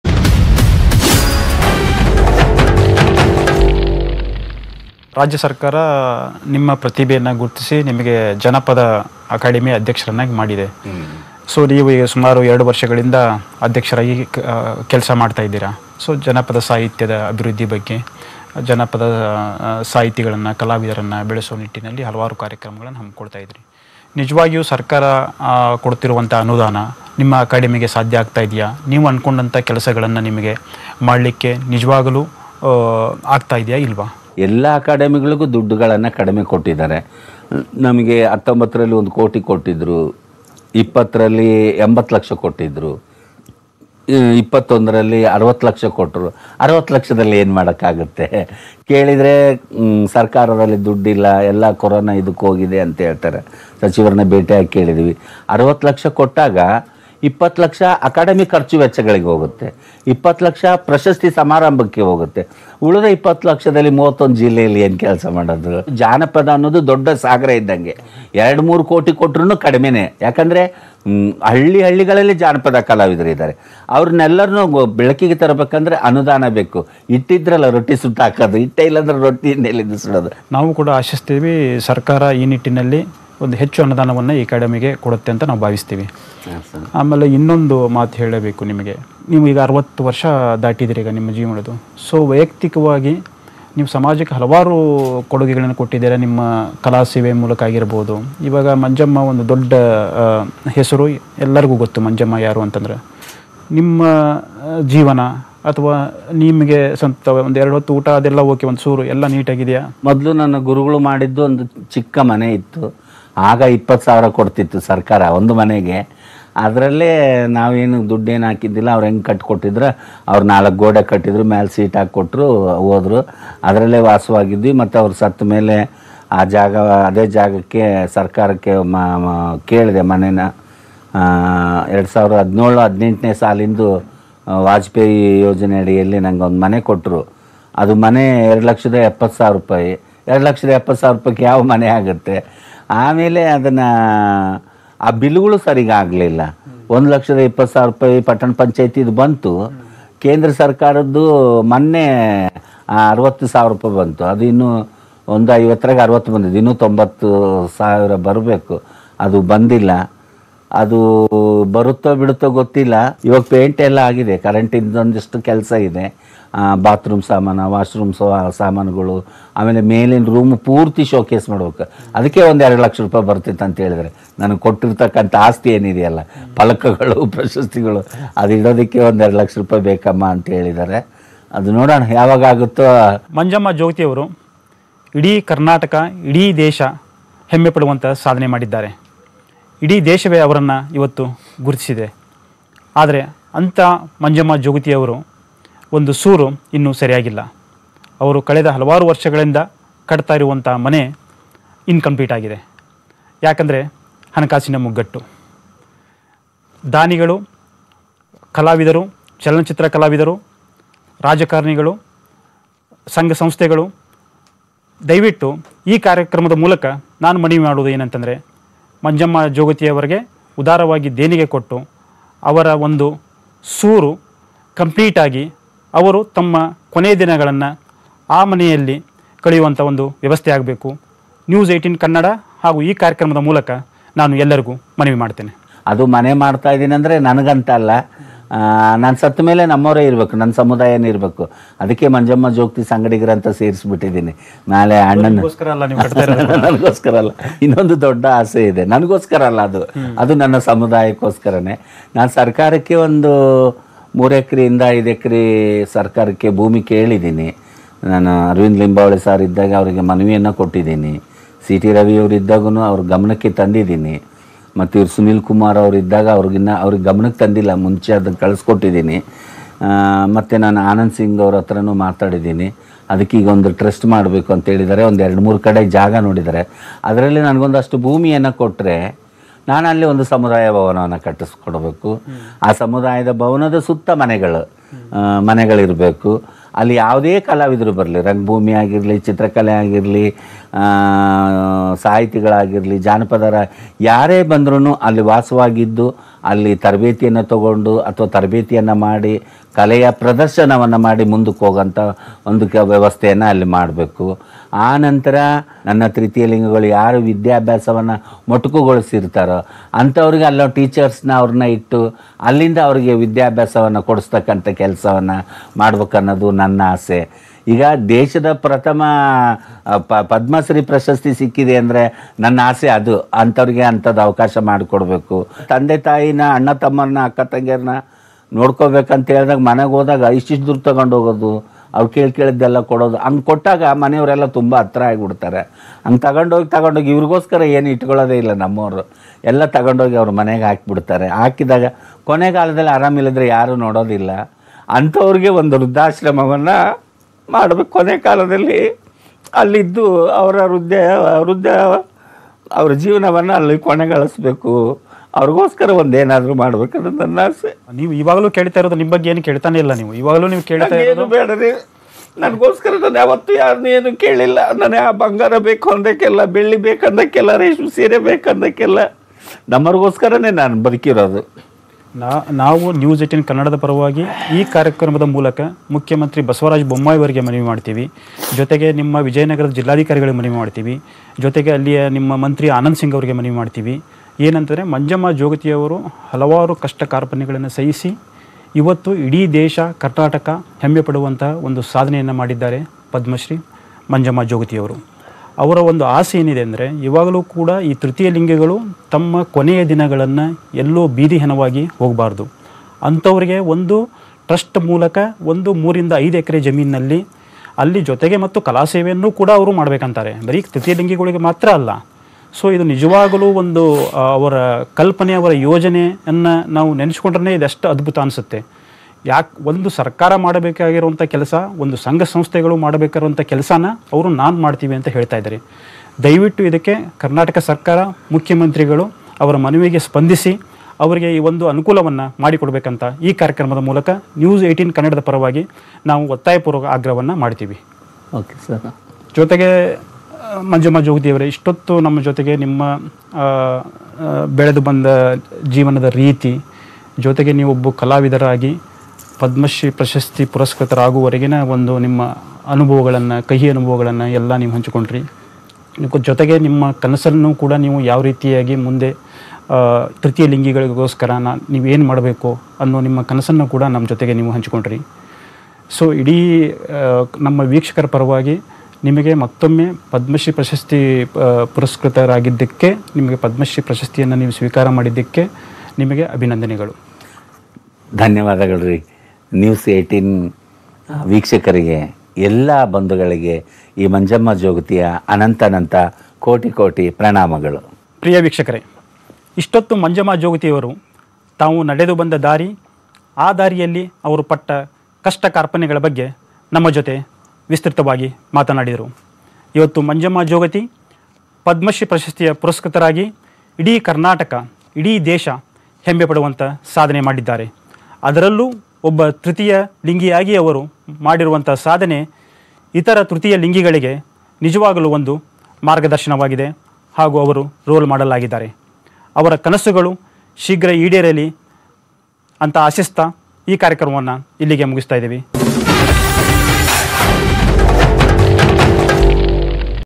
Manjamma Jogati Interview | ದೇಶ ಮೆಚ್ಚಿದ ಸಾಧಕಿ ಮಂಜಮ್ಮ ಸೂರಿನ ಚಿಂತೆ | Siddu Kaloji
Manjamma Jogati Interview |  ಕಲೆಯಿಂದ ಬದುಕು ಕಟ್ಟಿಕೊಂಡ ಜೋಗತಿ, ದೇಶ ಮೆಚ್ಚಿದ ಸಾಧಕಿ ಮಂಜಮ್ಮ ಸೂರಿನ ಚಿಂತೆ,ನನಸಾಗುತ್ತಾ ತೃತೀಯ ಲಿಂಗಿಯ ‘ಆ’ ಕನಸು | Siddu Kaloji